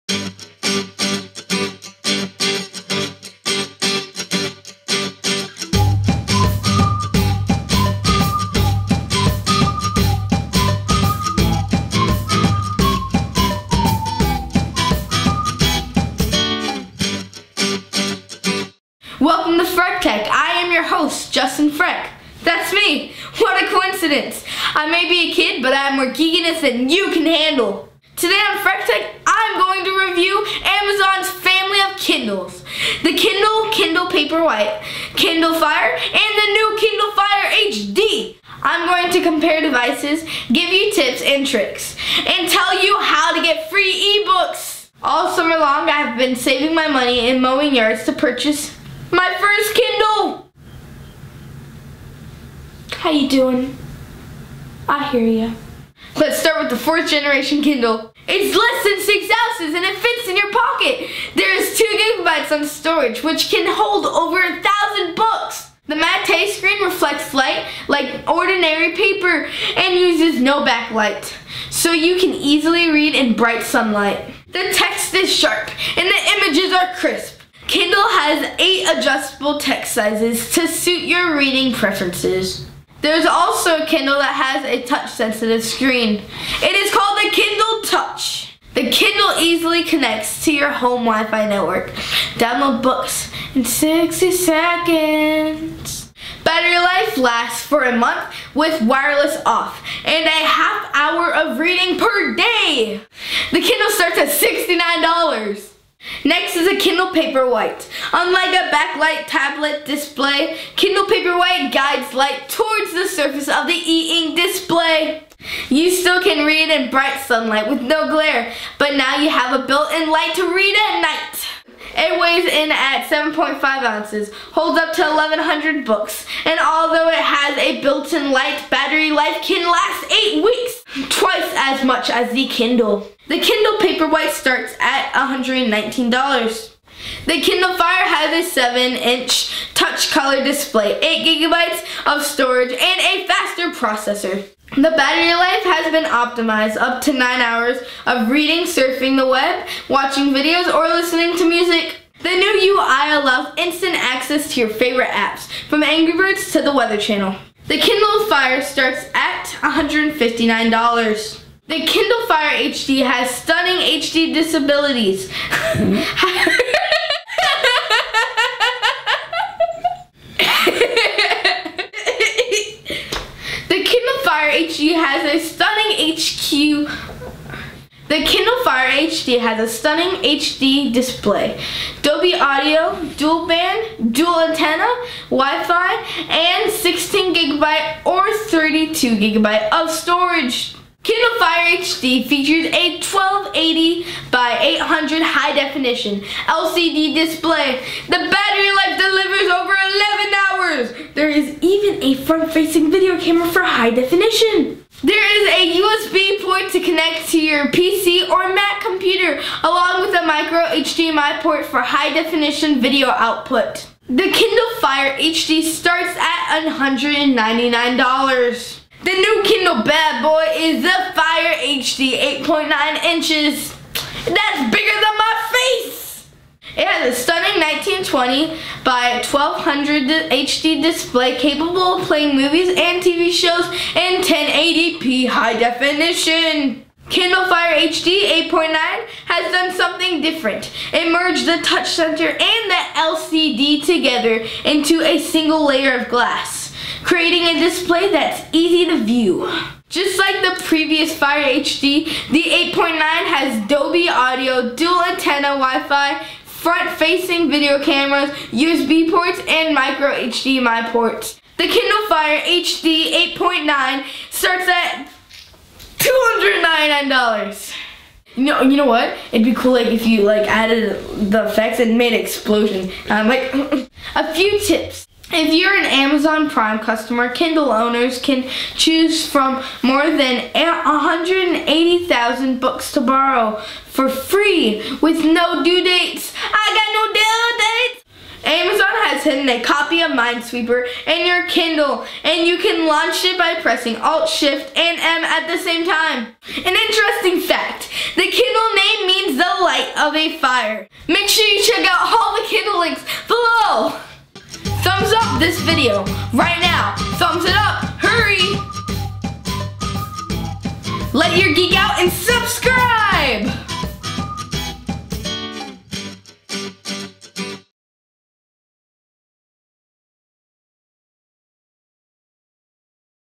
Comparison of Amazon Kindle Models - Frech Tech
I compare and review all the latest Amazon Kindle models, which include the Kindle, Paperwhite, Fire, Fire HD, and the new Fire HD 8.9".

Kindle Paperwhite
Kindle Fire
Kindle Fire HD
Kindle Fire HD 8.9"

Kindle owners who are Amazon Prime members can choose from more than 180,000 books to borrow for free with no due dates.